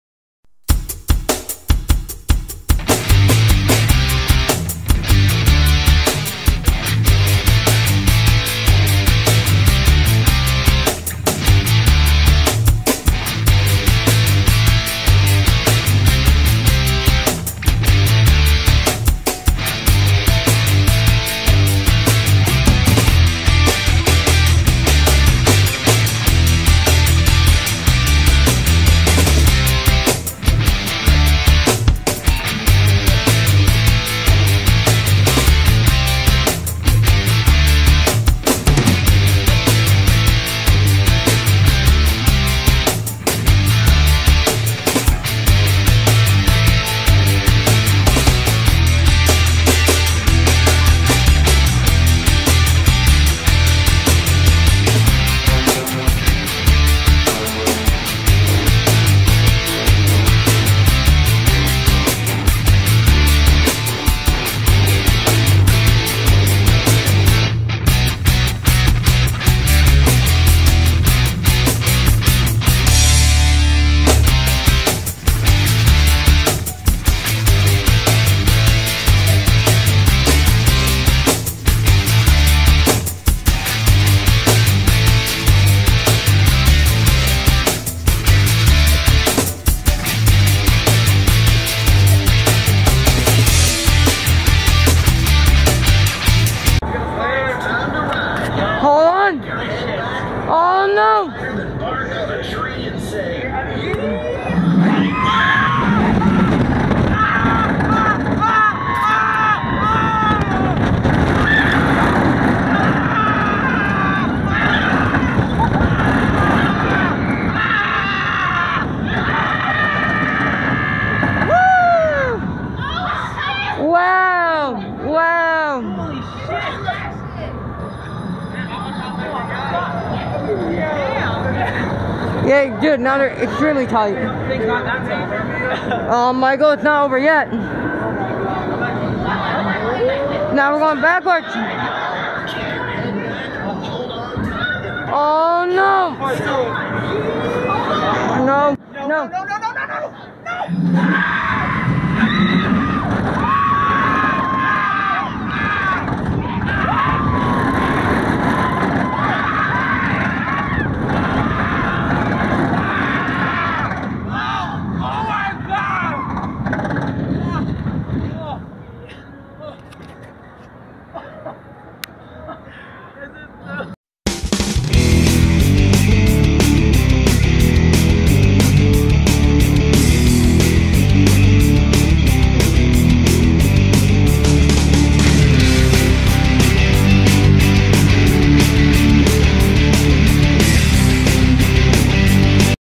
Sandy's Blasting Bronco (3rd Row POV)(Nickelodeon Universe)(NON-COPYRIGHT)
This is a one of a kind Intamin Launch coaster located at Nickelodeon Universe inside the American Dream Mall in the Meadowlands Complex in East Rutherford New Jersey. This POV was carefully shot with MingSung 1080p video recording sunglasses. POV taken February 17th, 2

0:00 Intro
1:37 POV
2:54 Outro